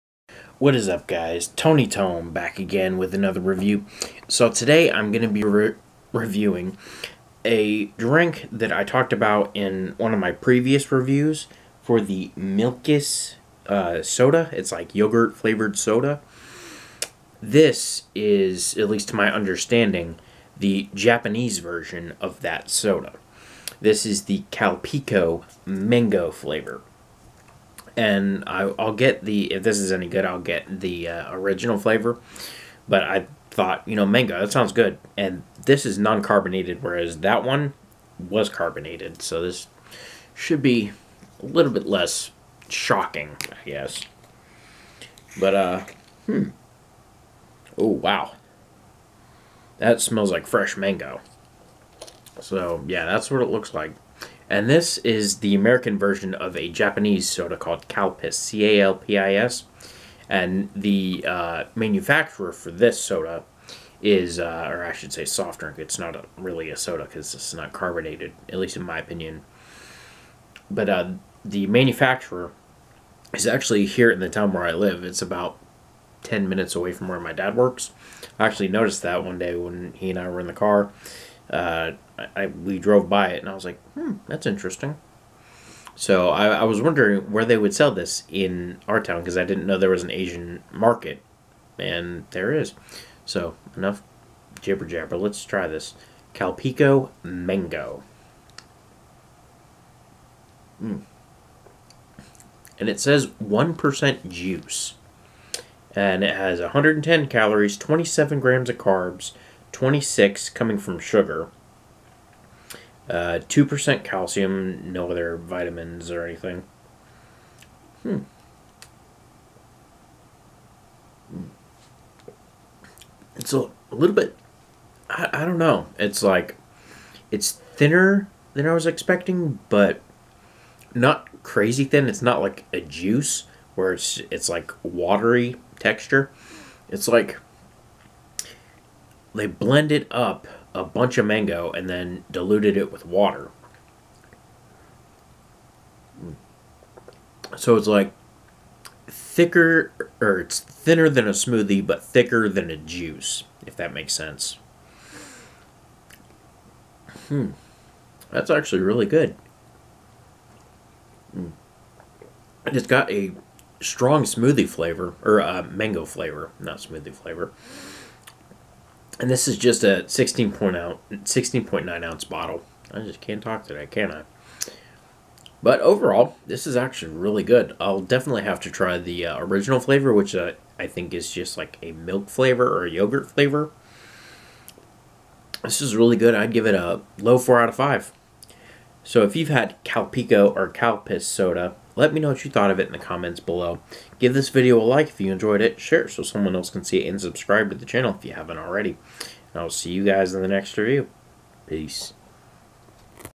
Calpico Mango Beverage
in this video I try a Japanese Smoothie Drink that i got at an Asian market in town